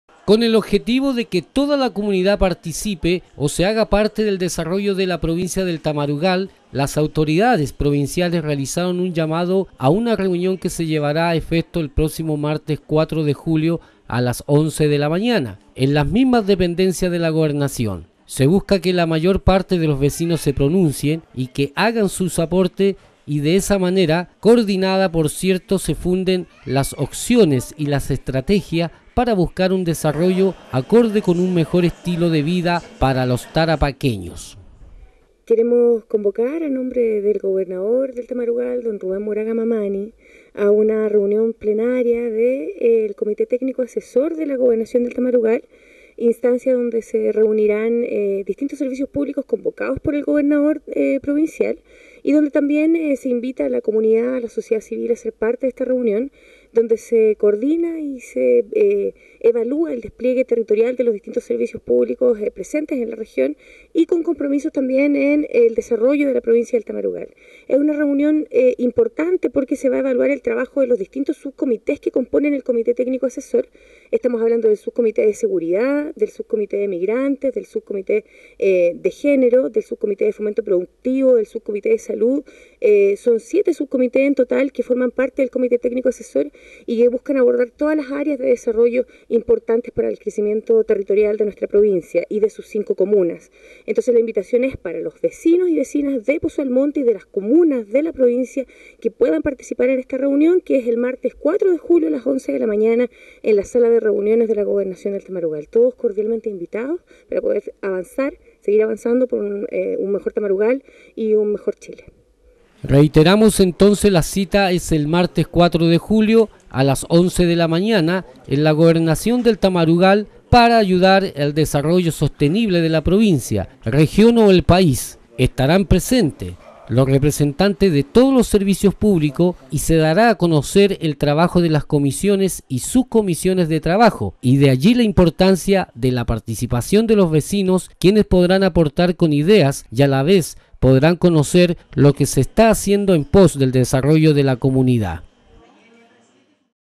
Servicios Públicos a disposición de la gente
Con el objetivo de que toda la comunidad participe o se haga parte del desarrollo de la Provincia del Tamarugal, las autoridades provinciales realizaron un llamado a una reunión que se llevará a efecto el próximo Martes 4 de Julio a las 11 de la mañana, en las mismas dependencias de la Gobernación.